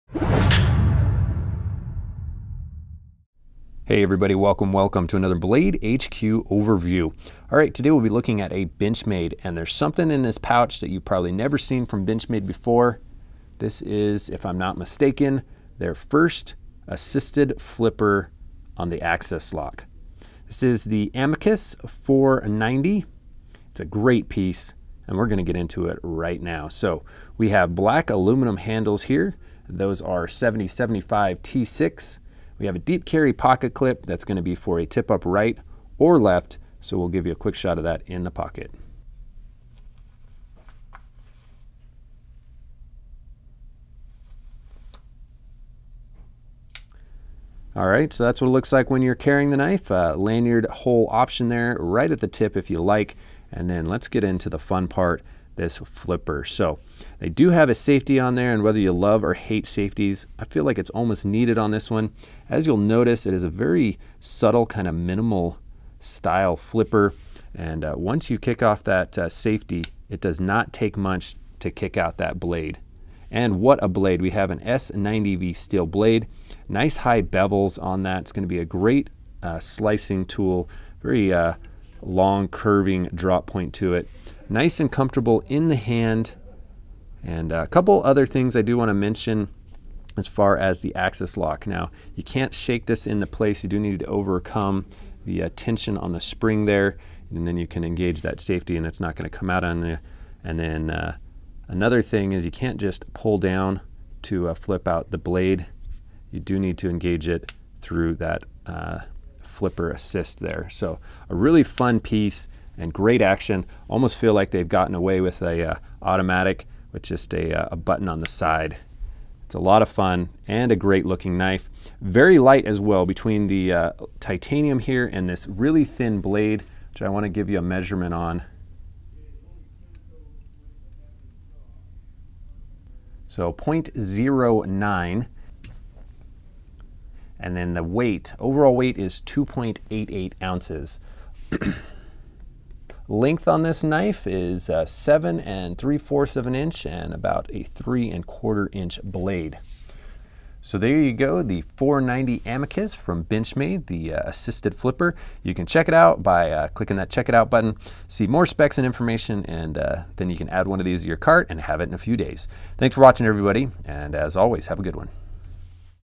Benchmade 490 Amicus Folding Knife Overview
Benchmade's Amicus 490 is the first AXIS-Assist knife to utilize a flipper opener! Designed with everyday carry comfort in mind, it has a thin, lightweight 7075-T6 aluminum handle with a deep carry, reversible pocket clip. For an added measure of protection, Benchmade has included a front-mounted safety lock. Adding even more draw to the Amicus knife, is the super premium CPM-S90V stainless steel blade.

The Amicus 490 has a satin finished blade with a plain cutting edge.